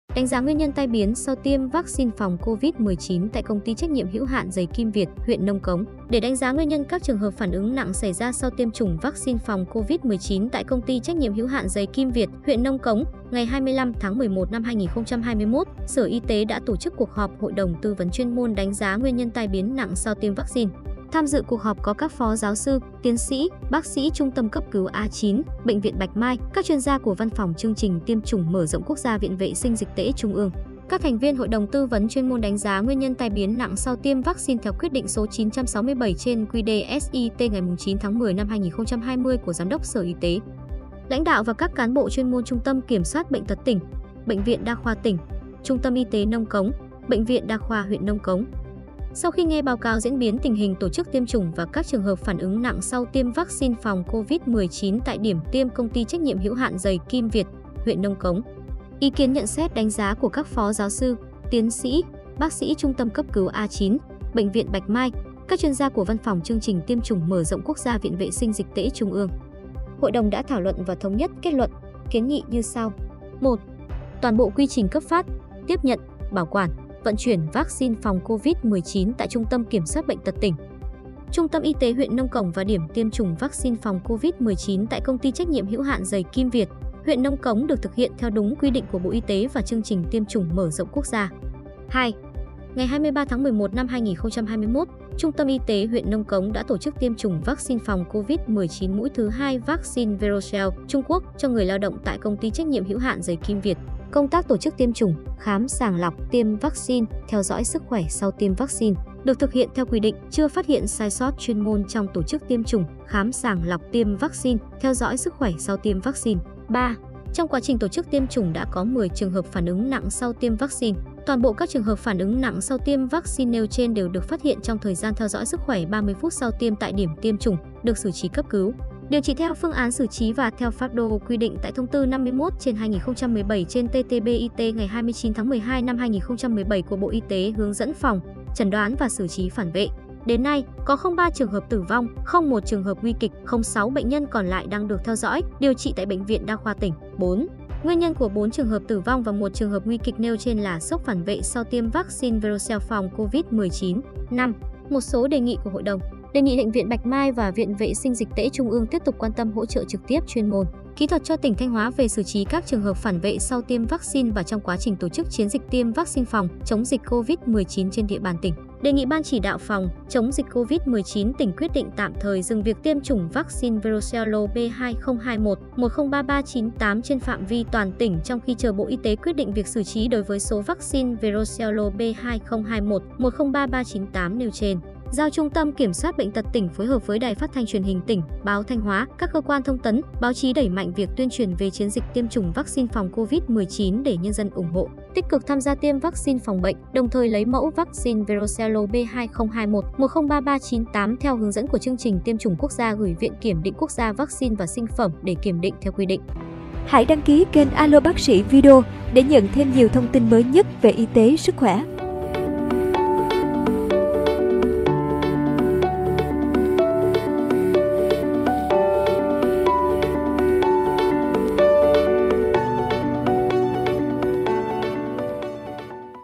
Đánh giá nguyên nhân tai biến sau tiêm vắc xin phòng COVID-19 tại Công ty TNHH giầy Kim Việt
Để đánh giá nguyên nhân các trường hợp phản ứng nặng xảy ra sau tiêm chủng vắc xin phòng COVID-19 tại Công ty TNHH giày Kim Việt, huyện Nông Cống; ngày 25/11, Sở Y tế đã tổ chức cuộc họp Hội đồng tư vấn chuyên môn đánh giá nguyên nhân tai biến nặng sau tiêm vắc xin.

Cập nhật diễn biến dịch COVID-19 mới nhất:
MỌI GÓP Ý XIN GỬI VỀ:
Hợp tác nội dung:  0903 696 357